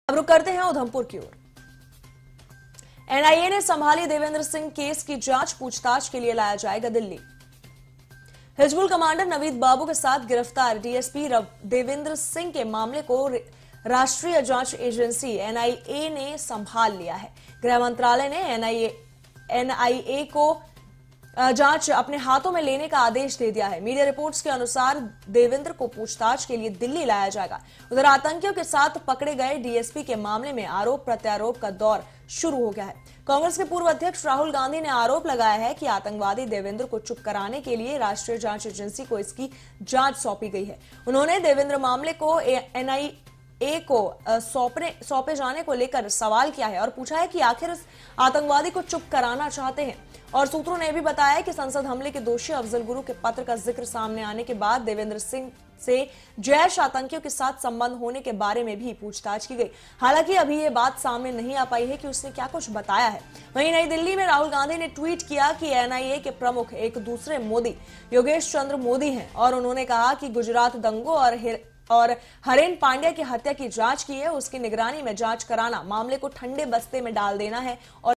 Udhampur एनआईए ने संभाली दविंदर सिंह केस की जांच, पूछताछ के लिए लाया जाएगा दिल्ली
हिजबुल कमांडर नवीद बाबू के साथ गिरफ्तार डीएसपी दविंदर सिंह के मामले को राष्ट्रीय जांच एजेंसी (एनआईए) ने संभाल ली है। गृह मंत्रालय ने एनआईए को जांच अपने हाथों में लेने का आदेश दिया था। मीडिया रिपोर्ट्स के अनुसार दविंदर को पूछताछ के लिए दिल्ली लाया जाएगा। उधर, आतंकियों के साथ पकड़े गए डीएसपी के मामले में आरोप-प्रत्यारोप का दौर भी शुरू हो गया है। कांग्रेस के पूर्व अध्यक्ष राहुल गांधी ने आरोप लगाया कि आतंकवादी दविंदर को चुप कराने के लिए राष्ट्रीय जांच एजेंसी (एनआईए) को इसकी जांच सौंपी गई है। उन्होंने दविंदर मामले को एनआईए को सौंपे जाने को लेकर सवाल किया कि आखिर कौन इस आतंकवादी को चुप कराना चाहता है? सूत्रों ने बताया कि संसद हमले के दोषी अफजल गुरु के पत्र का जिक्र सामने आने के बाद दविंदर सिंह से जैश आतंकियों के साथ संबंध होने के बारे में भी पूछताछ की गई।